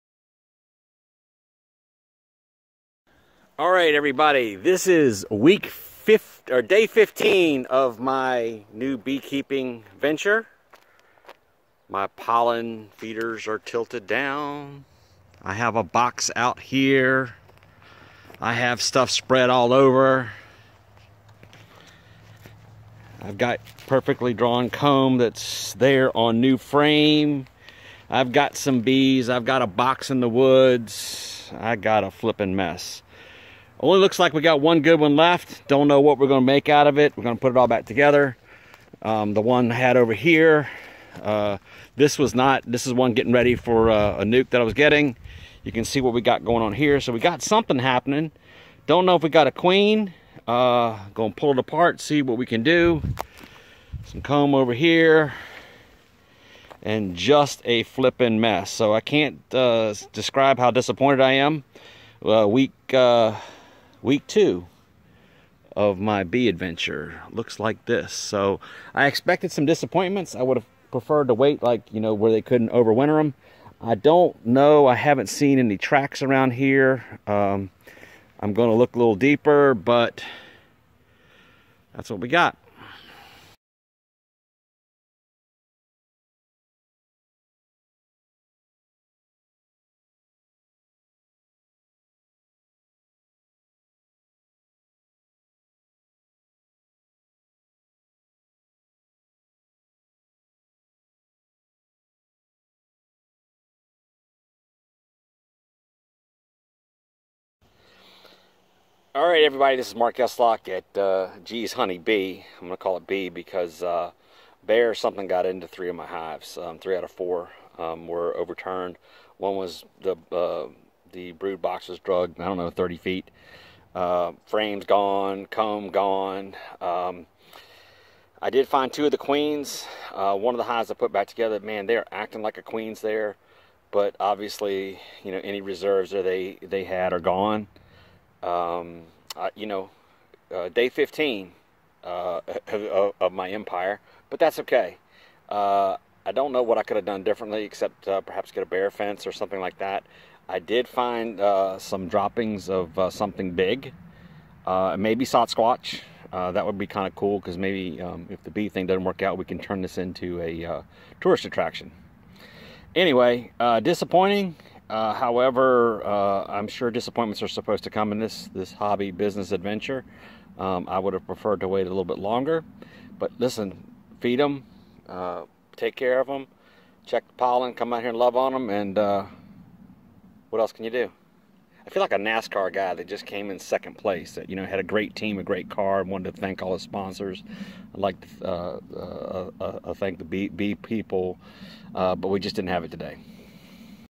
Day 15 of Gees Bees. Bear Visit
This was Day 15 of my first year of beekeeping. The hives are located in Orange VA 2019